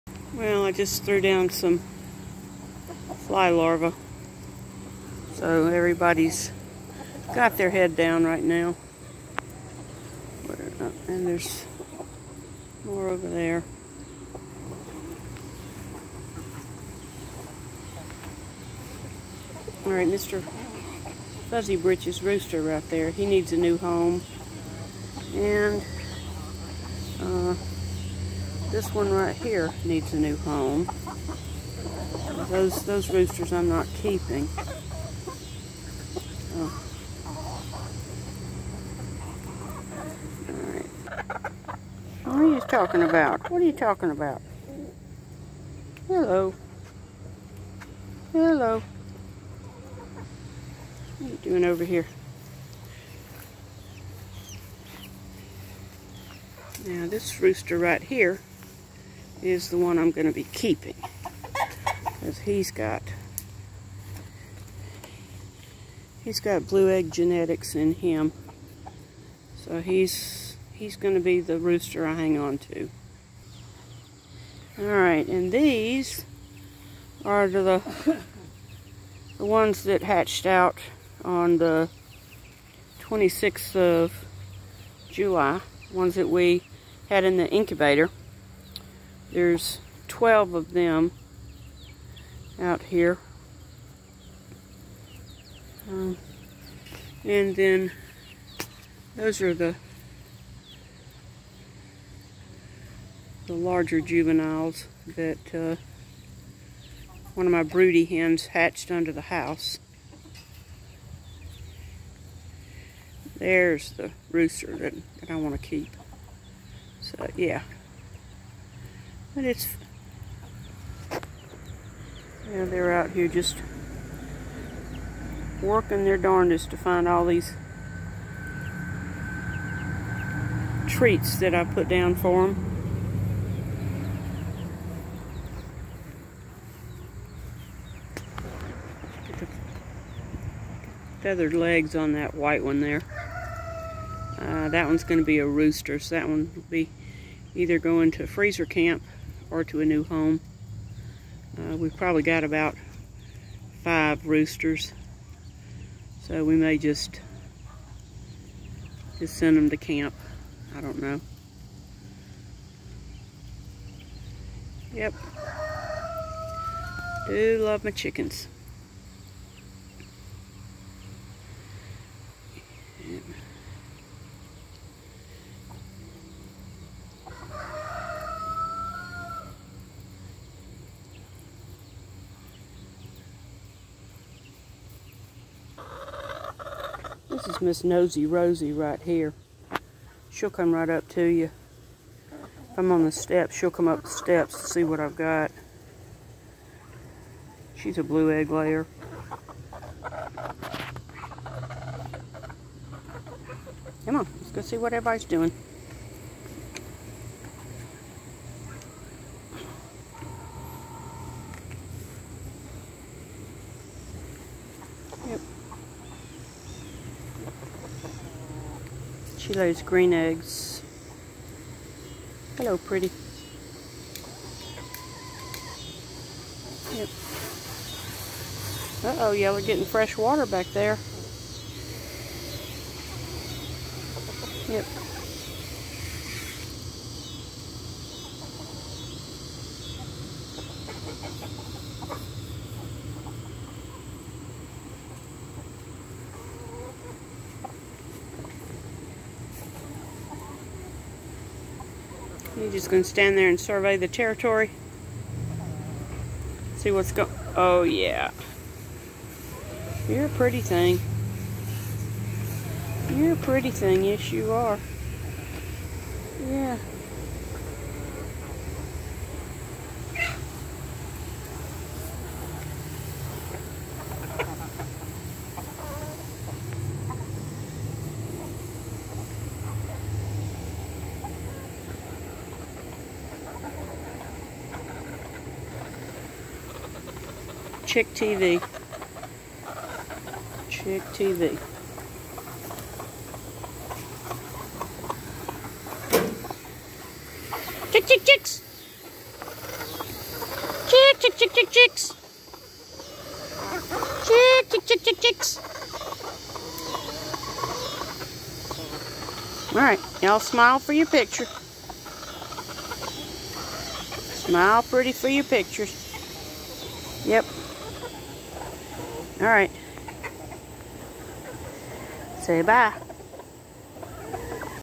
Checking in on the Chickens
I threw down some Fly Grubs for the chickens late on Friday afternoon.  I thought I might be able to get a count on how many I actually have out there; but that didn't happen! But they really enjoyed their treats!